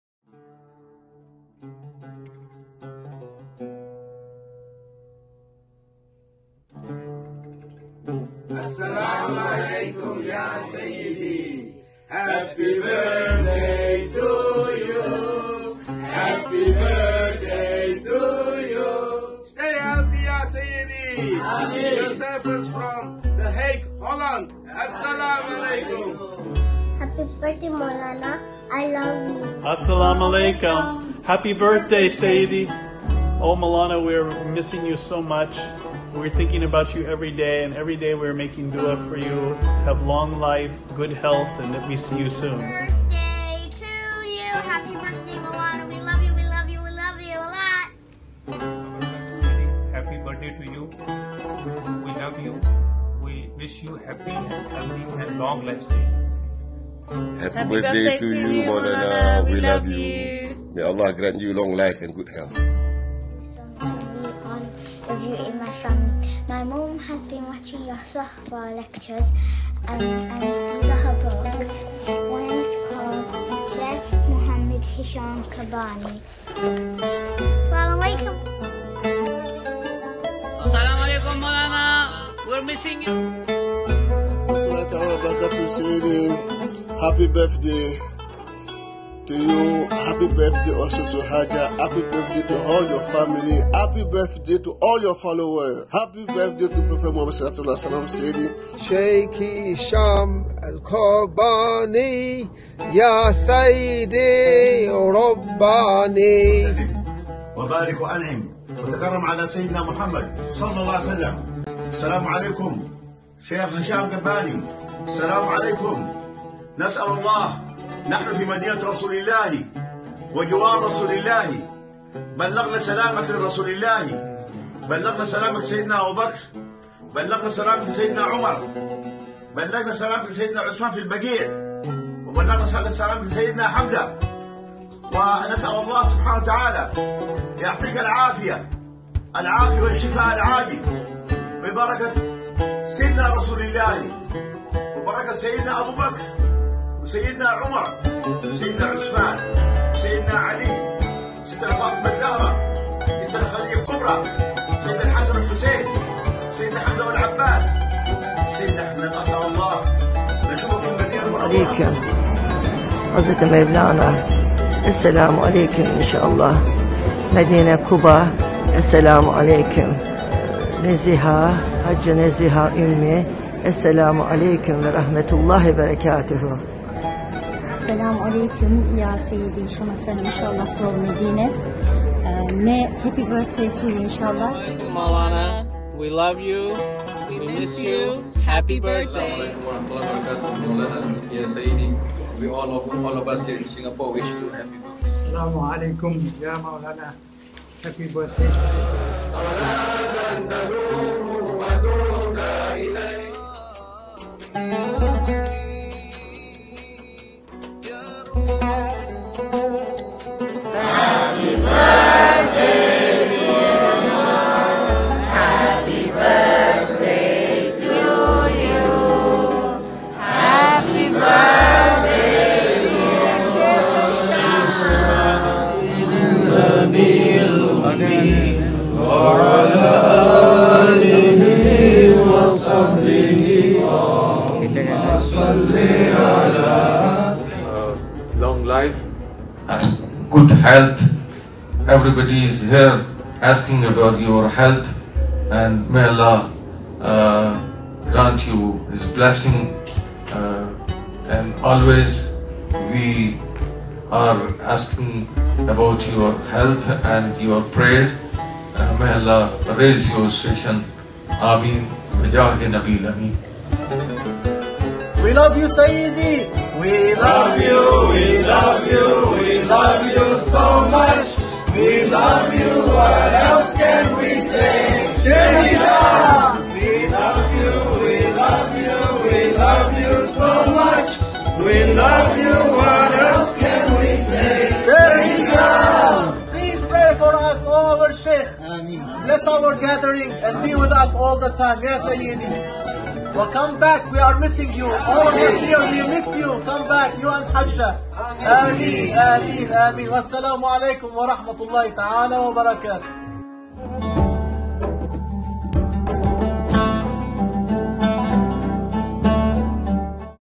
Happy Birthday Mawlana Shaykh Hisham Kabbani, part 2
A video montage of birthday greetings from around the world from lovers of Mawlana Shaykh Hisham Kabbani.